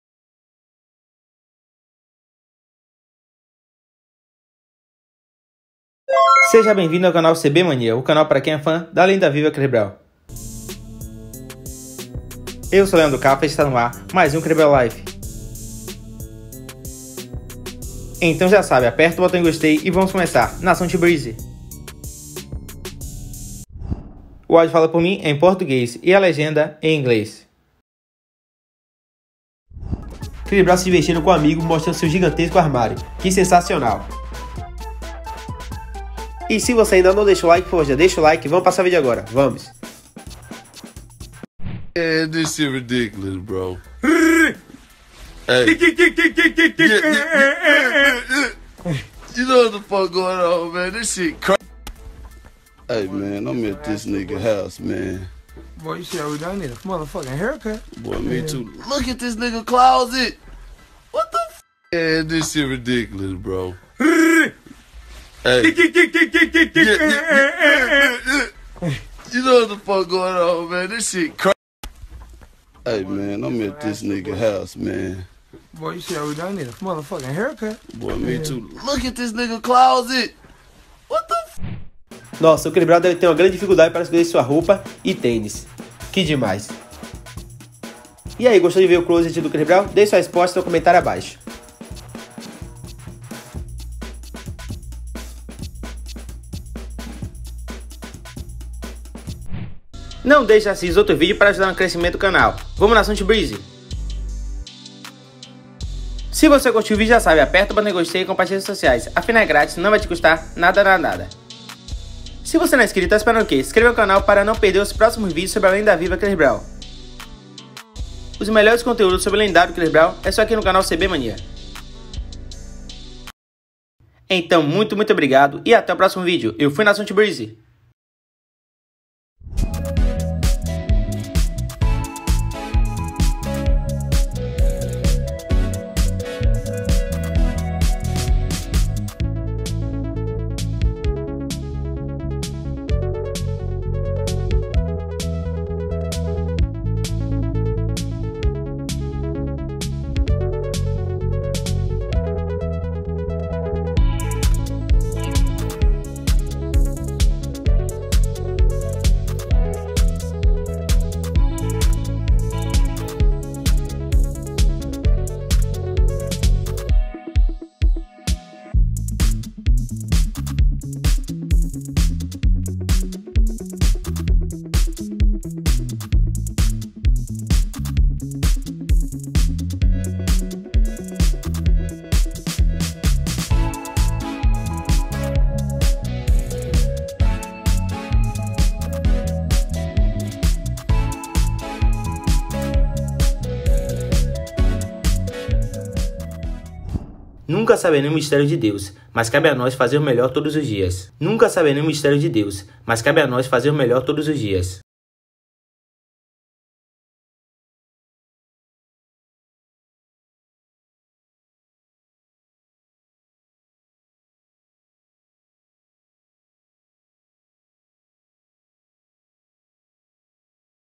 Chris Brown Makes Fun Of Shows Off His Gigantic Closet!
Chirs browns acting like desiigner while showing off his enormous closet. 😱🔥🤯